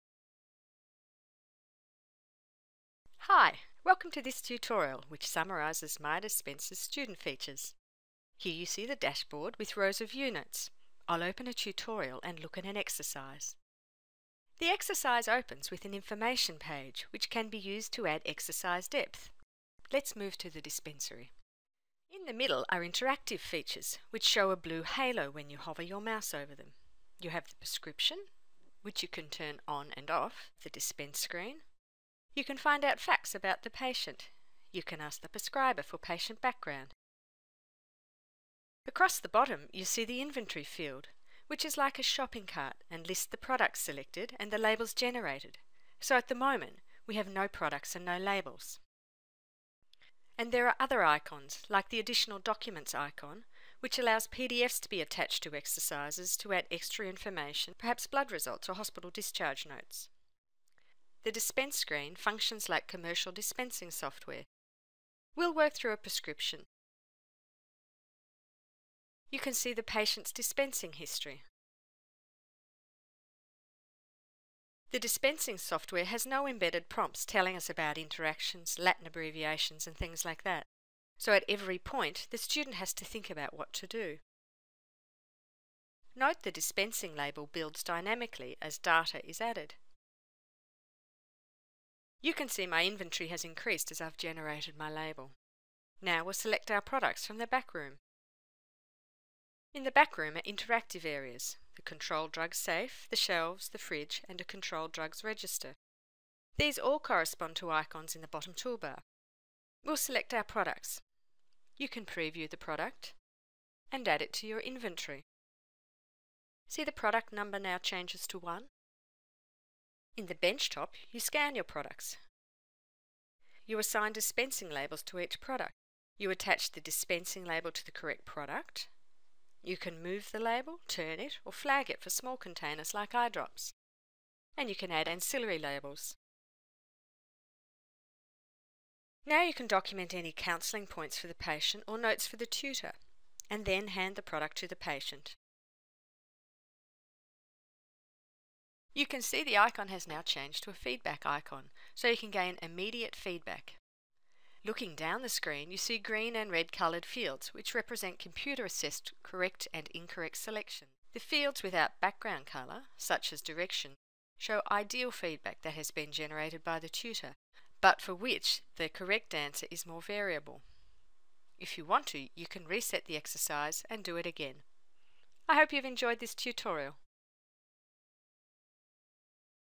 MyDispense: Working through an exercise
This video shows working through a MyDispense student exercise, and gives a guided overview of the software features that are seen.